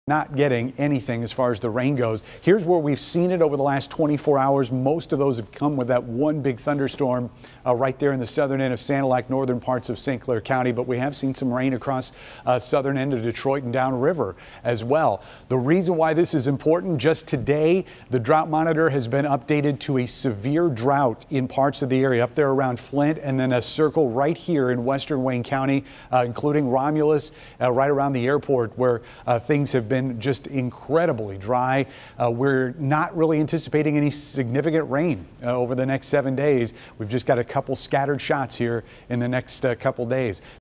Ben Bailey breaks down severe drought
Ben Bailey breaks down the severe drought hitting southeast Michigan during this dry summer.